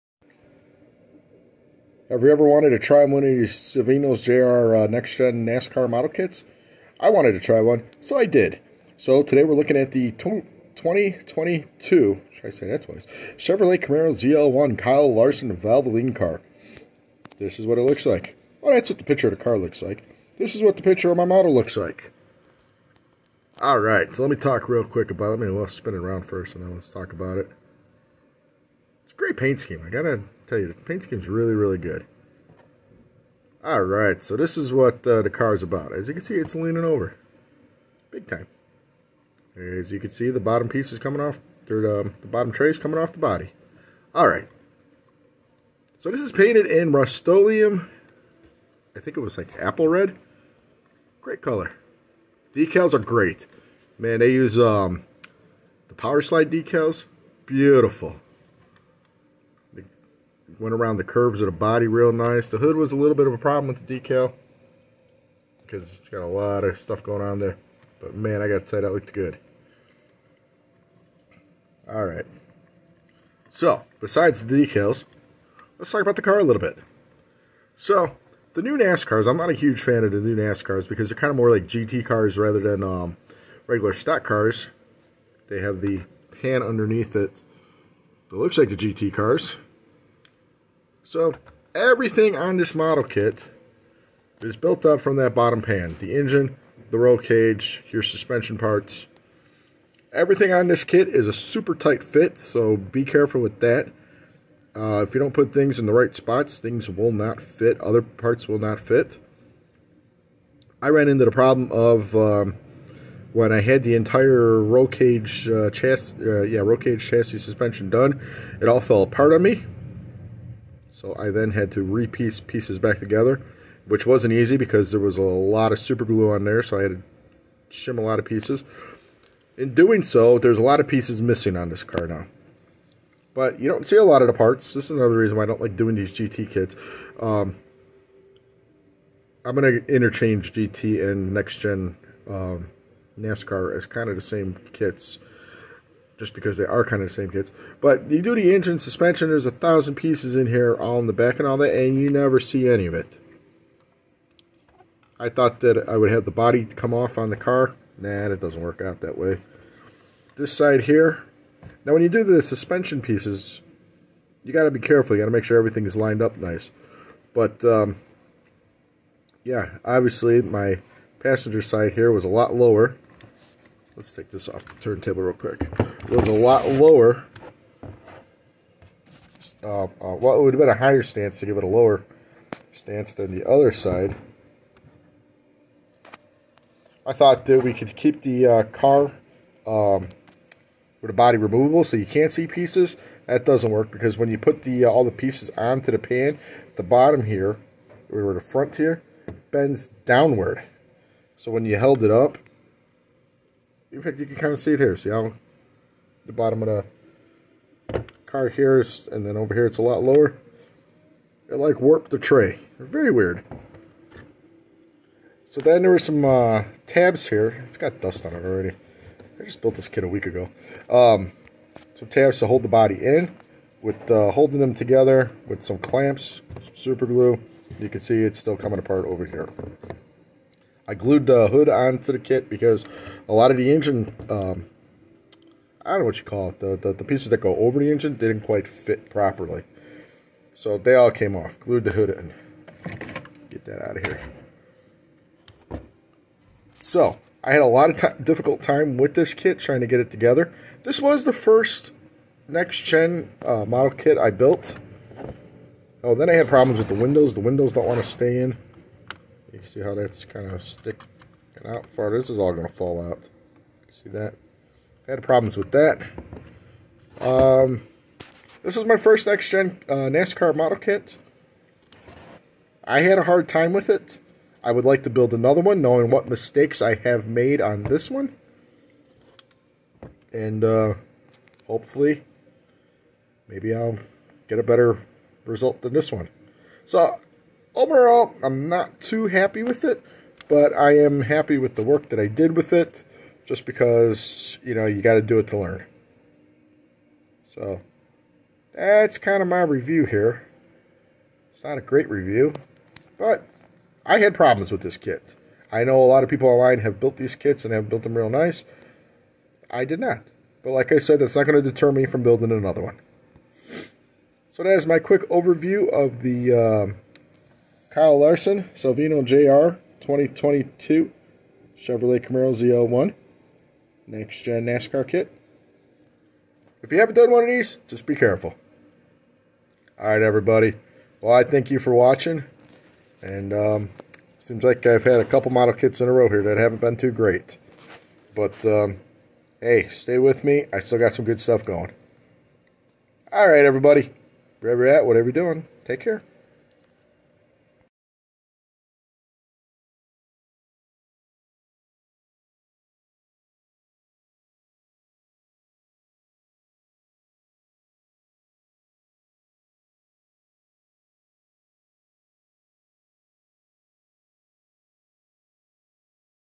My First NASCAR Next-Gen Build - Salvino Jr Models 2022 Kyle Larson Valvoline Chevrolet Camaro ZL-1
roadrunners "model car building"